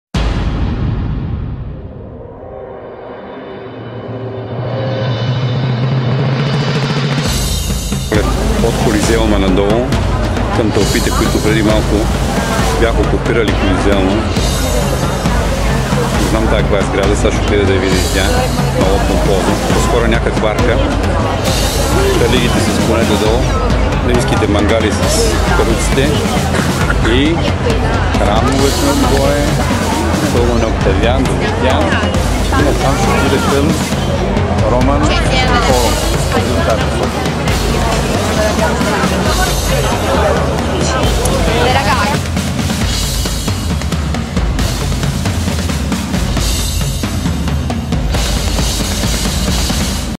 Колизеума в Рим,Италия с Даниел Димитров
Колизеума е не само сграда..тои е символ на една епоха и една империя...